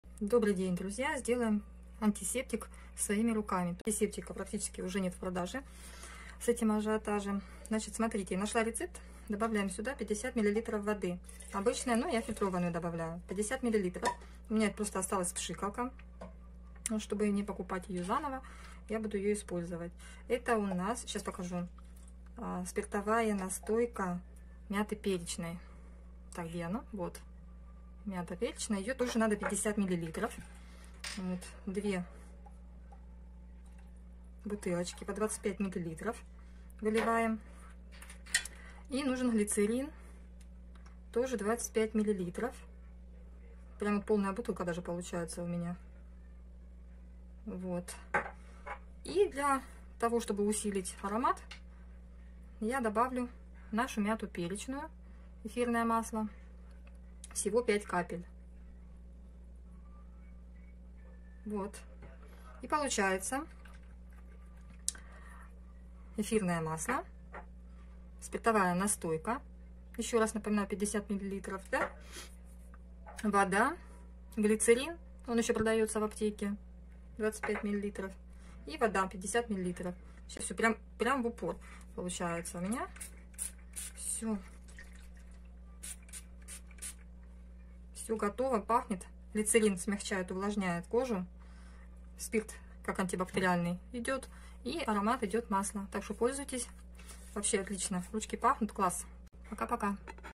Антисептик своими руками с маслом Greenway
Спасибо 🌺 партнёру нашей команды Академиков за идею 👍